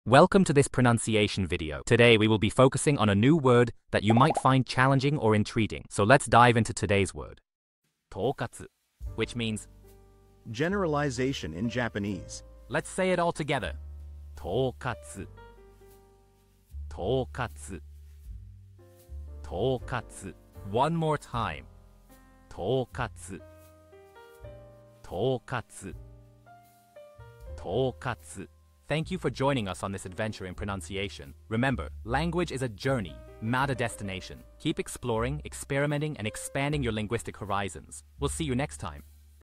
How to pronounce toukatsu | 統括 (Generalization in Japanese)
Master the Pronunciation of 'toukatsu | 統括' - which means : Generalization in Japanese 📝 with @PronunciationJapanese 🎓🗣️ - [Your Guide to Japanese]

Learn to pronounce 'toukatsu' perfectly in Japanese with our easy and engaging tutorial by @PronunciationJapanese! 📖📽️ In this comprehensive video, we provide you with native speaker examples 🗣️👂 and phonetic breakdowns 🎵 to ensure you get the pronunciation just right.